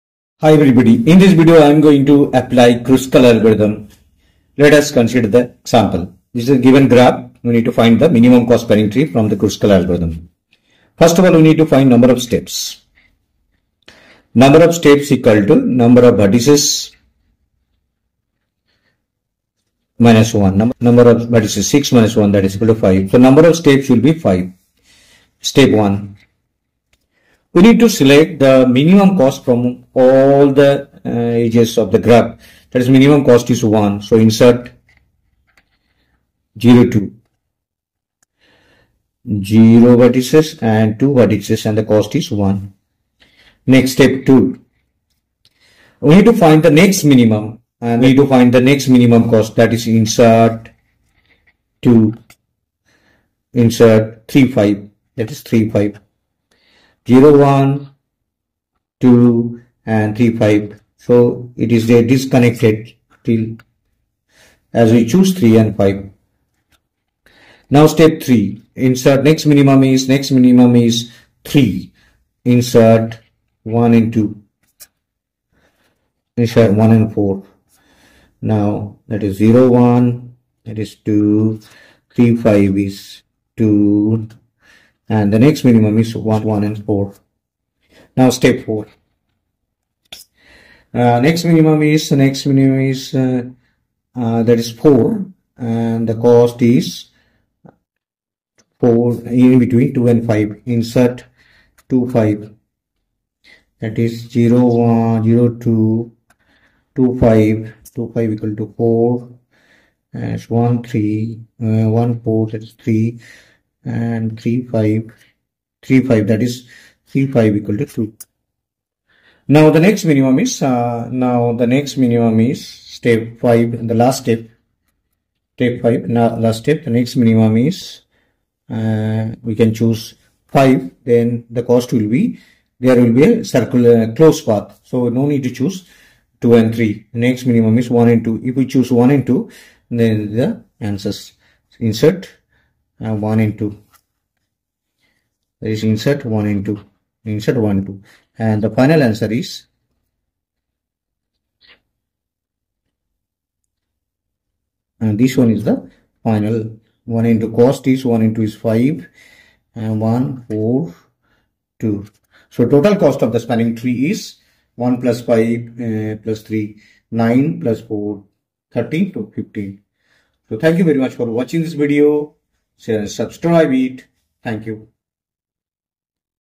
Kruskal’s Algorithm Explained with Example | Minimum Spanning Tree Made Easy
WhiteboardExplained (if it's animated or whiteboard-based)

DSA (for Data Structures and Algorithms)